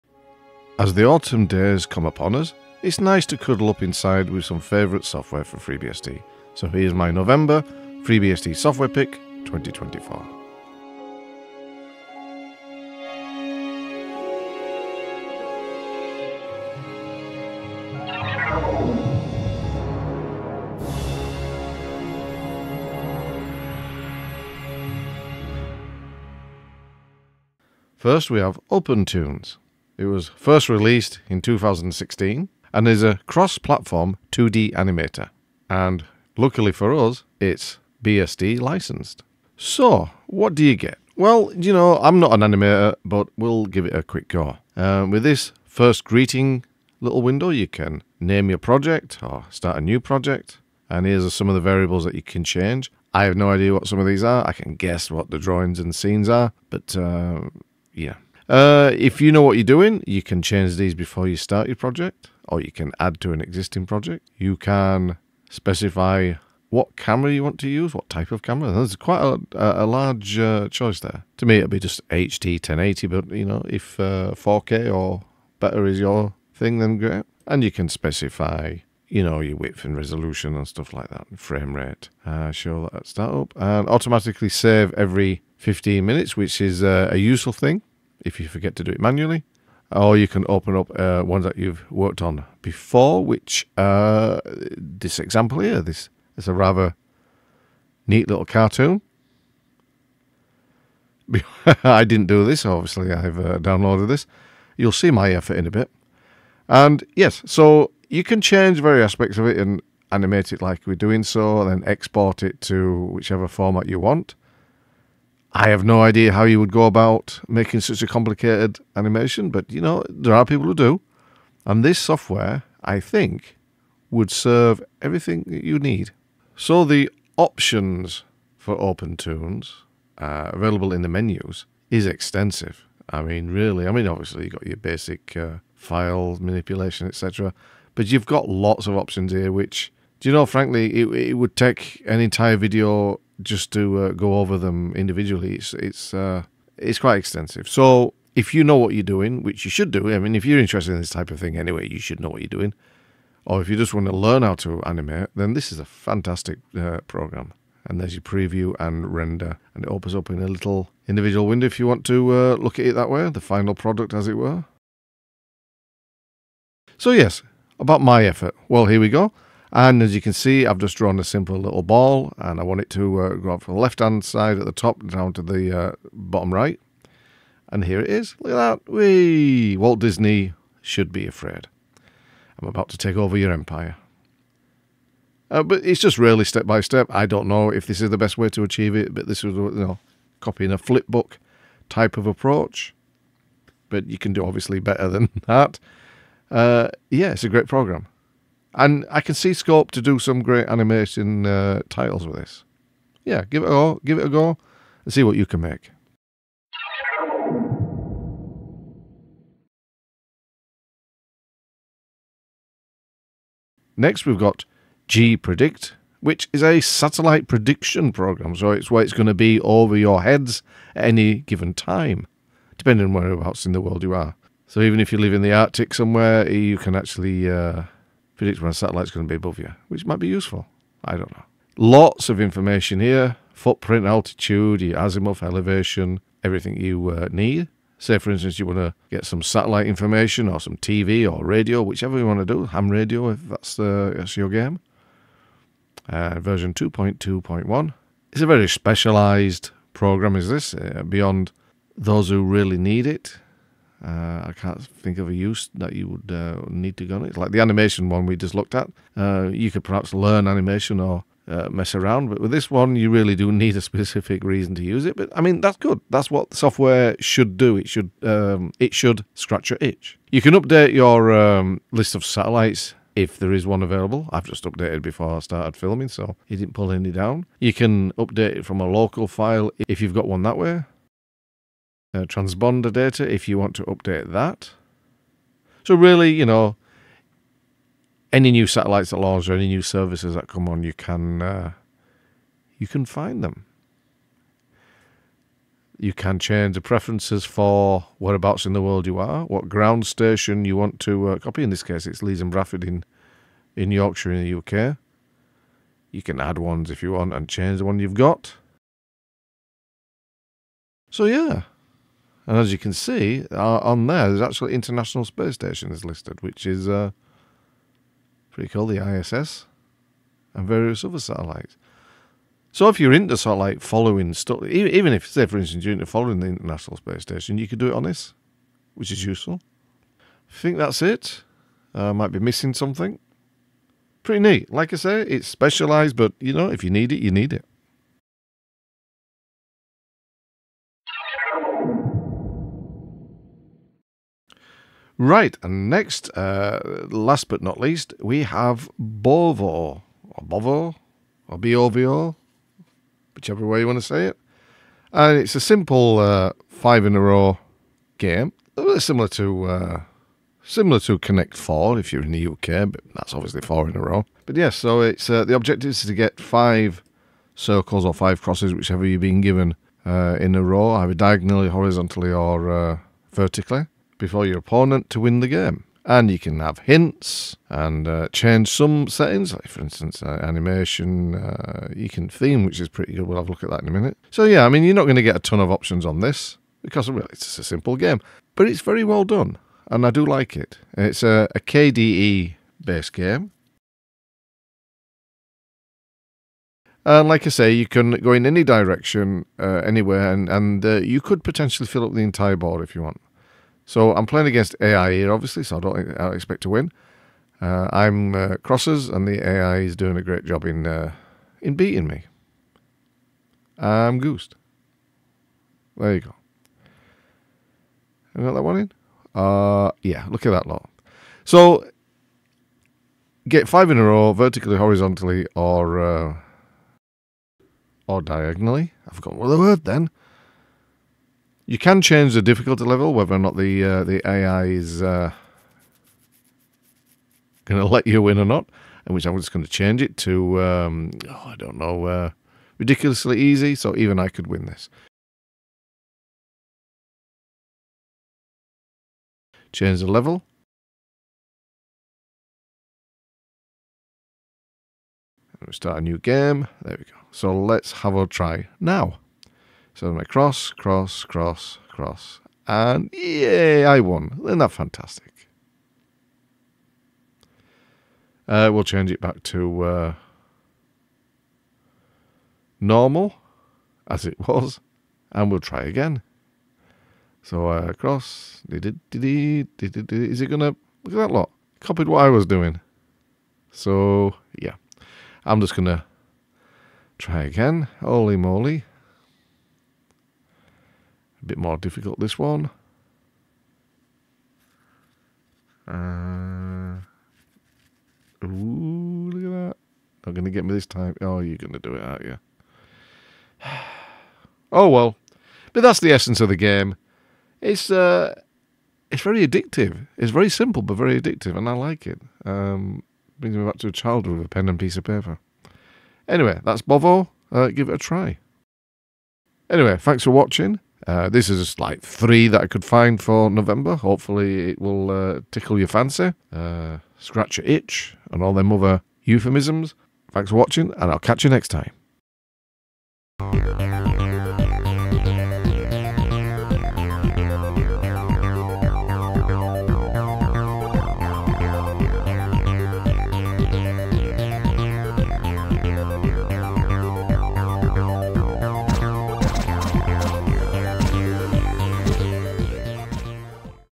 FreeBSD Software Pick November 2024
There is so much software available in the FreeBSD repository that sometimes gems can go unnoticed... so in this monthly series we'll highlight some of these interesting & useful programs.
In this video we present 3 such gems...
Timestamps:
0:00 - Intro
0:18 - OpenToonz
3:26 - GPredict
5:41 - Bovo